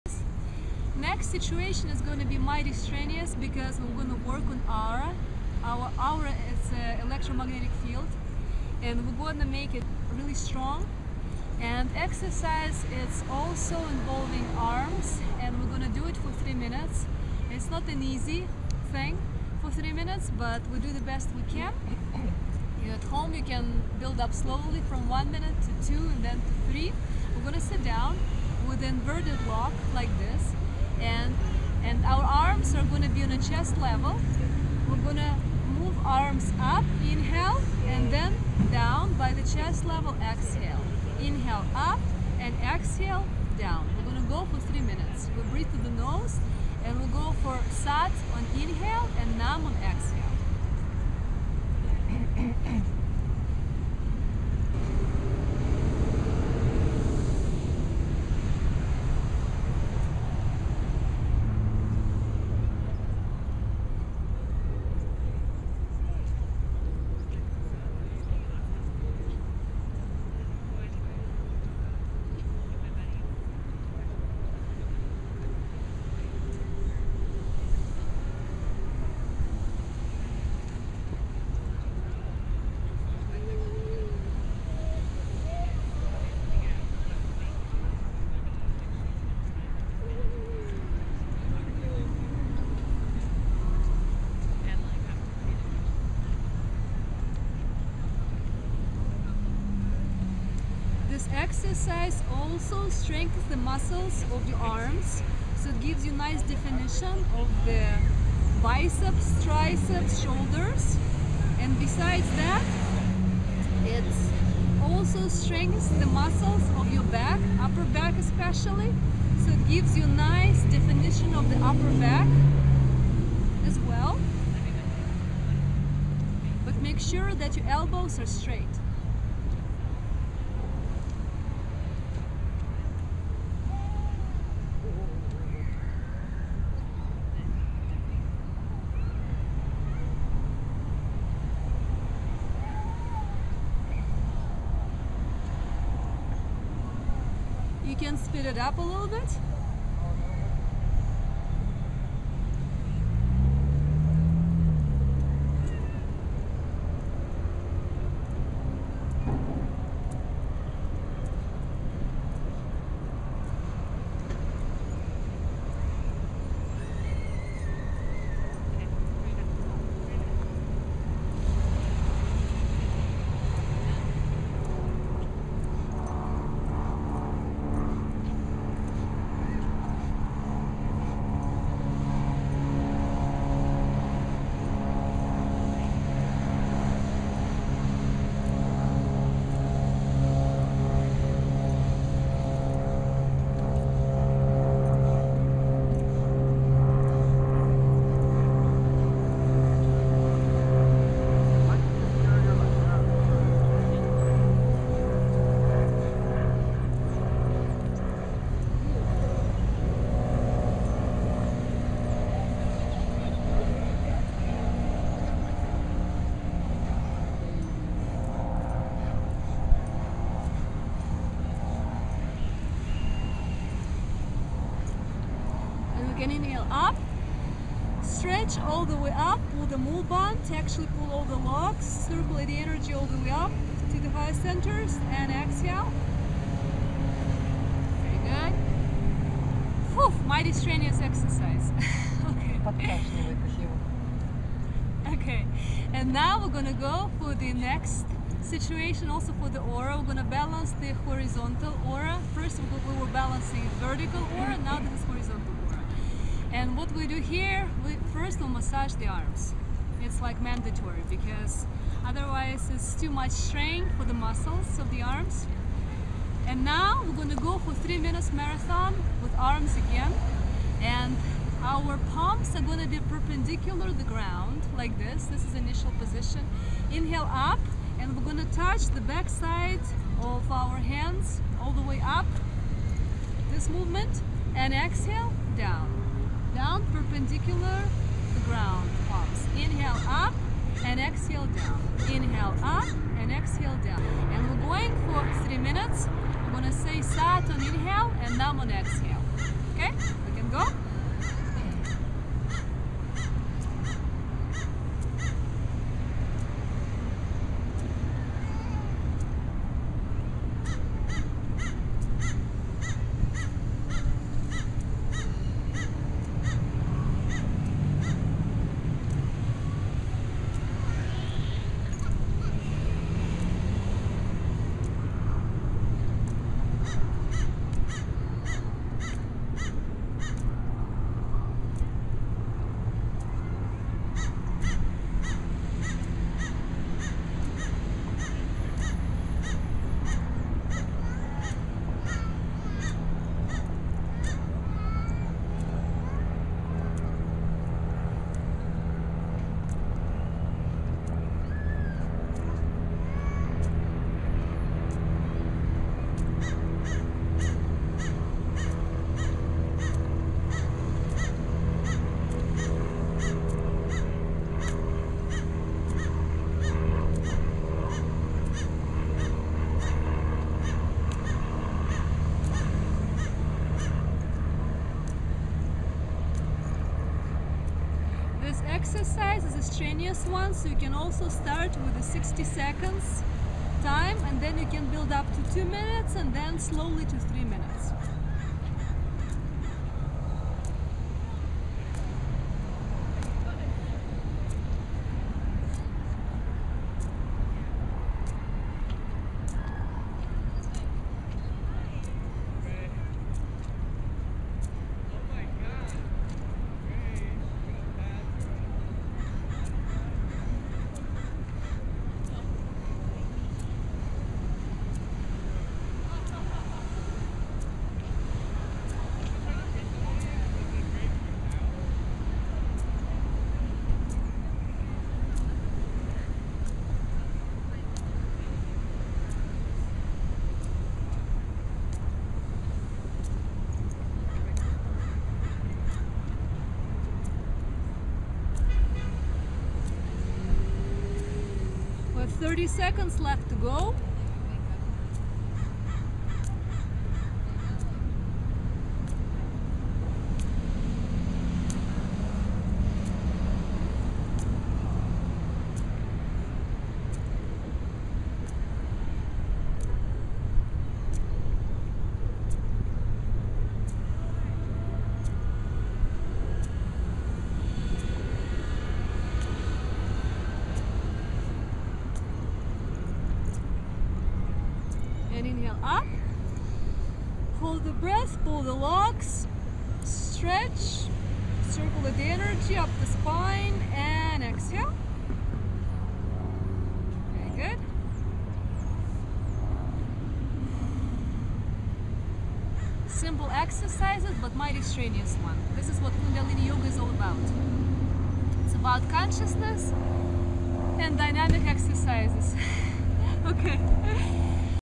PRANAYAMA FOR AURA - part 2
"PRANAYAMA" COMPLEX - part 2 AURA BALANCING SET
Vertical & Horizontal Aura balance - for inner peace, serenity, clarity & certainty + good set for upper body shape. Continue with part - 3

КUNDALINI YOGA/YOGA EXERCISES TO HELP RECUP FROM RADIATION EFFECTS:
- aura boost: exercises for aura + BOF + long meditations (2.5 hours)
- immune system boost: BOF; 4th Chakra exercises - thymus gland; 1st chakra exercises;
- enocrine system boost: meditations; 6th chakra exercises; concentration on 3d eye point; mantras decitations - stimulation of hipothalamus gland; exercises for head centers: 6th, 7th chakras;
- thyroid gland boost: 5th chakra - neck exercises;
- blood purification: BOF, chlorophil +
- limphatic system boost - arm exercises, leg lifts/sriss cross leggs-arms; greap fruit /garlic /lemon / clove / orange cocktail;
- kidneys: set for kidneys;
- liver: torso twists;
- detox: yogi tea, green tea, lot's of water.